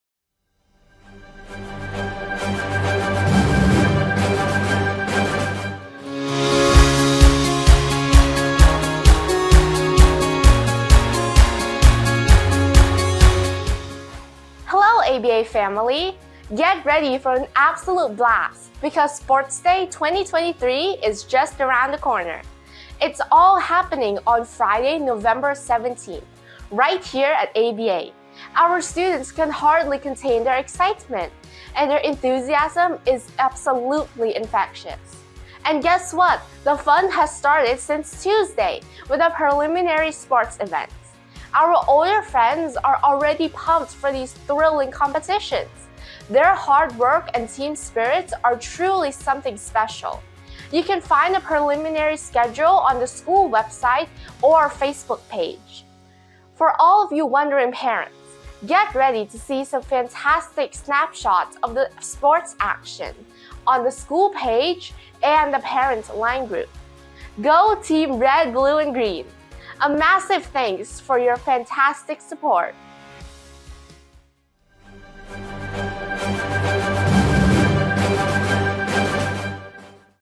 ABA Breaking News EP.13: Sports Day 2023 (Kelly G8)
🔆 Start stretching and get ready to cheer for the future champions! 🏆
Join us for the Ambassador Bilingual Academy Sports Day on November 16 - 17, 2023. It's a day of teamwork, determination, and unforgettable moments as our students showcase their athletic prowess. Don't miss this exciting event! 🌟🏅